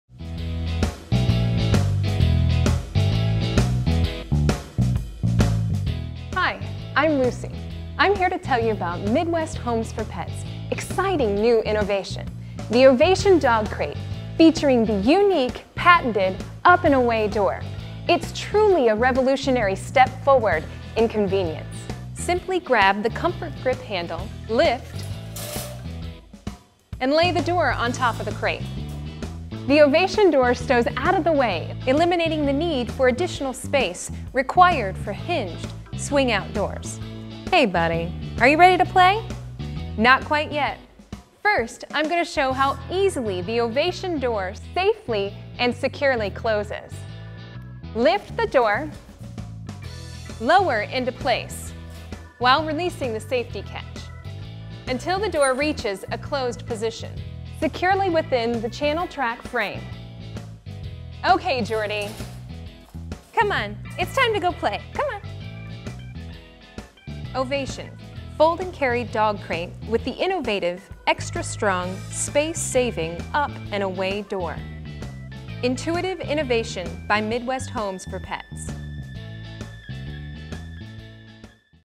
Ovation Single Door Crate with Up & Away Door - Extended
New Ovation Single Door Dog Crate from MidWest Homes for Pets, featuring the unique, space-saving, Up & Away Door.